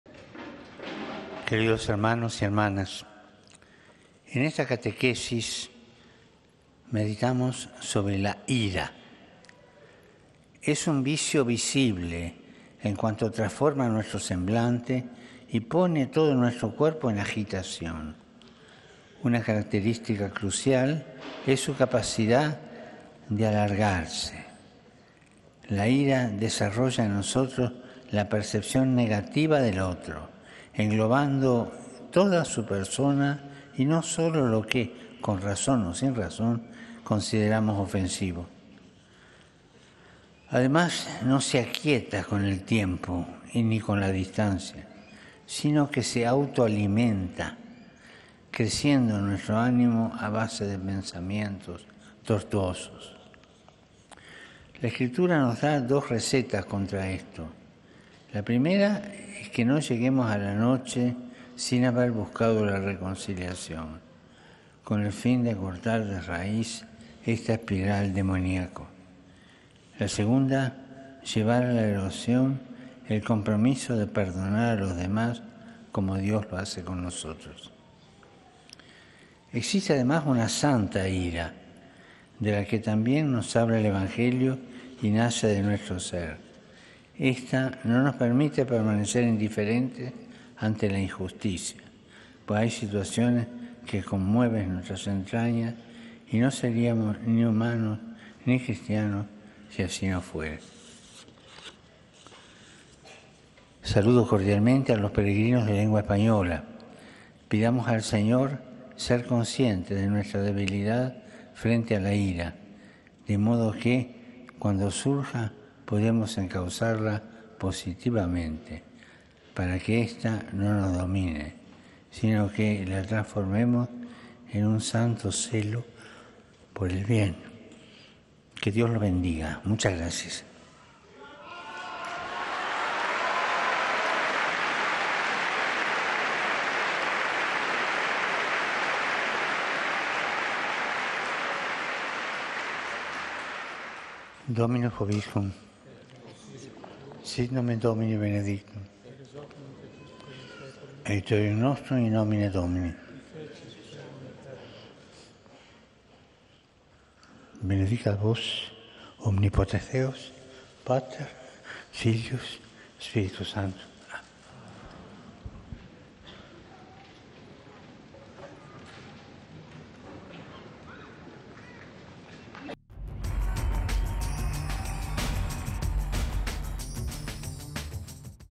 AUDIENCIA GENERAL | Francisco propone dos “recetas” para luchar contra el vicio de la ira
En la audiencia general, Francisco habló del vicio de la ira, uno de los más “visibles” del ser humano. Para luchar contra ella, ofreció dos “recetas” recogidas en la Biblia.